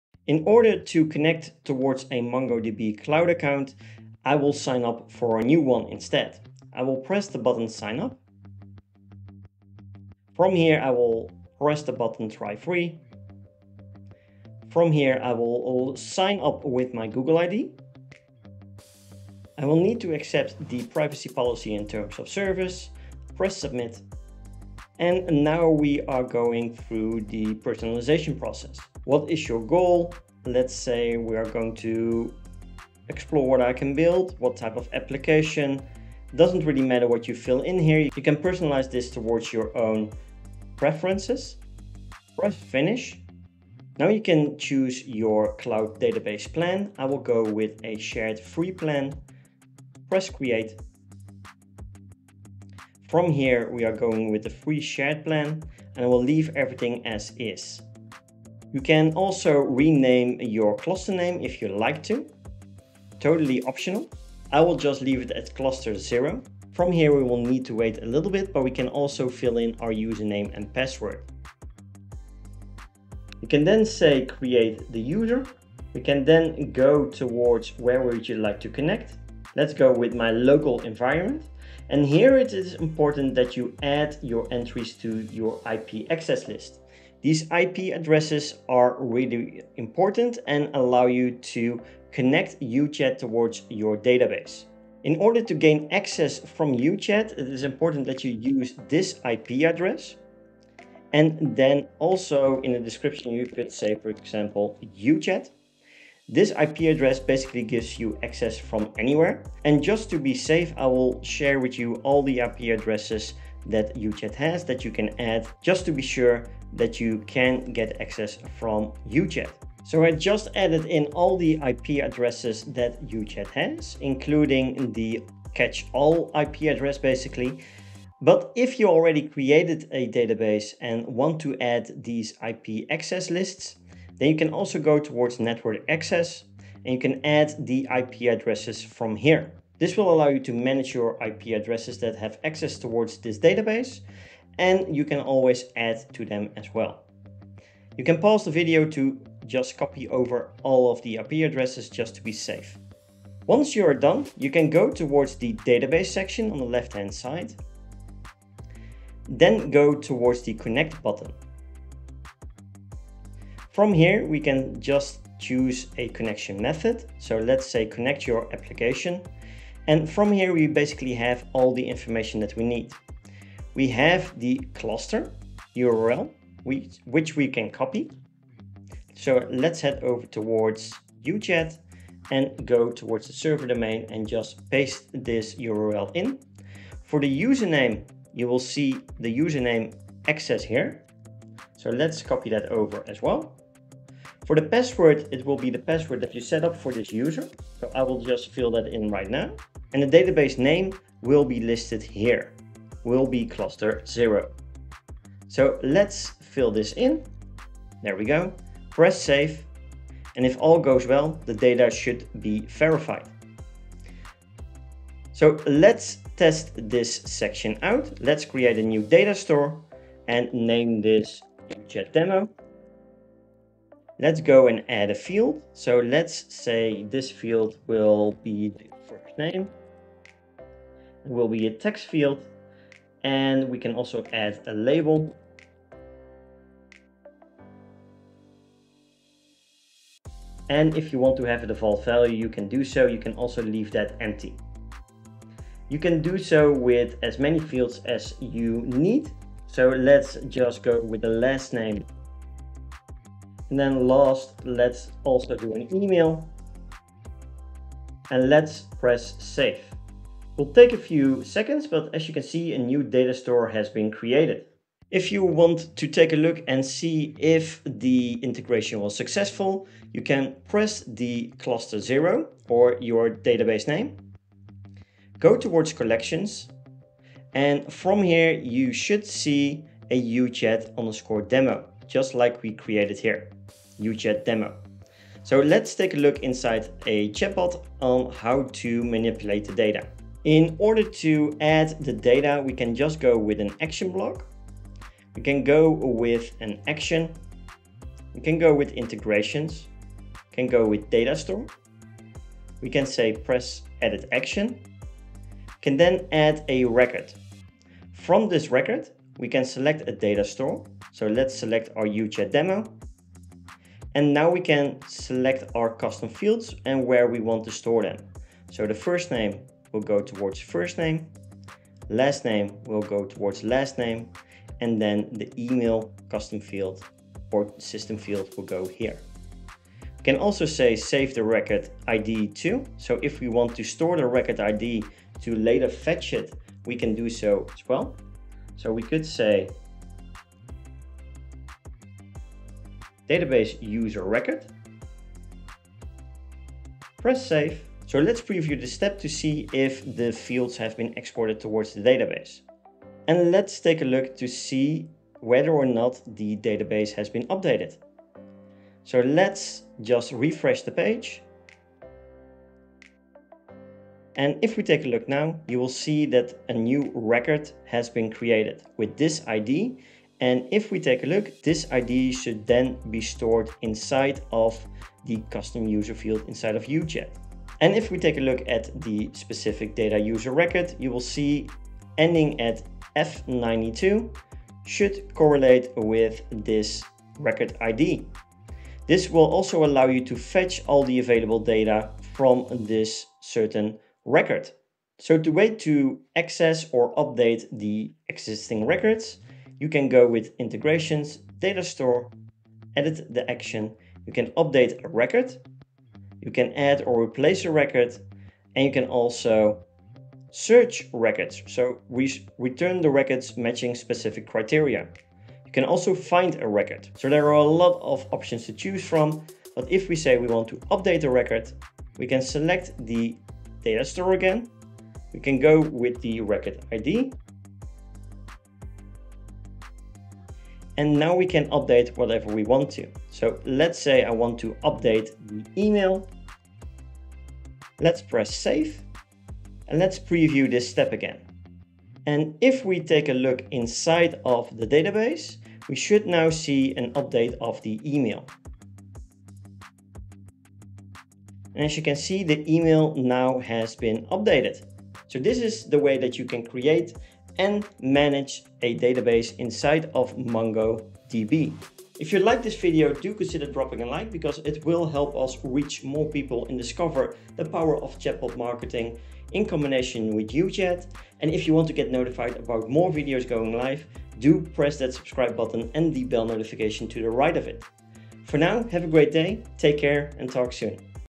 How to create and manage a MongoDB database for free
If you are wanting to create and manage a database (for free) then using MongoDB is a fantastic option and even better UChat provides a native integration!

In this video we show you step by step how to set it all up